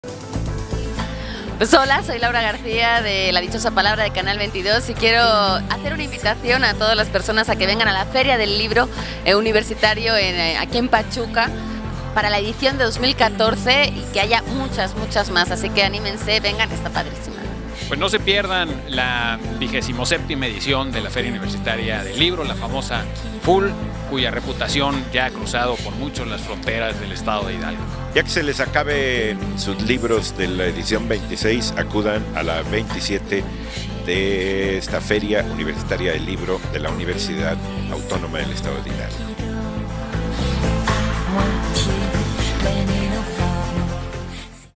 Los protagonistas del programa "La Dichosa Palabra" de canal 22, te invitan a la FUL 2014 UAEH
Los protagonistas del programa "La Dichosa Palabra" de canal 22,  te invitan a participar en la 27ª Feria Universitaria del  Libro, del 22 al 31 de agosto próximo en el Polideportivo Carlos Martínez Balmori, Pachuca, Hgo.